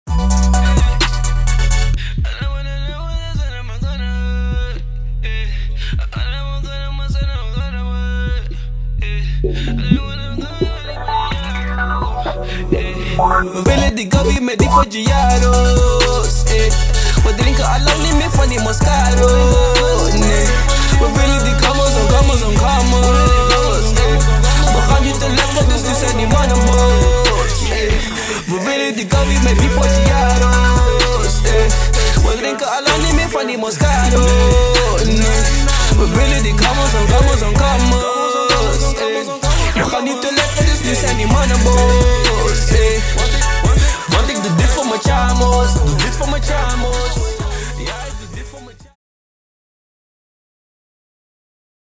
DRIVAH | Encore Fest 2015 | PHOTO RECAP
Checkout the vibes I provided during my set @ Encore Festival 2015...

Shot by: Antonio Photography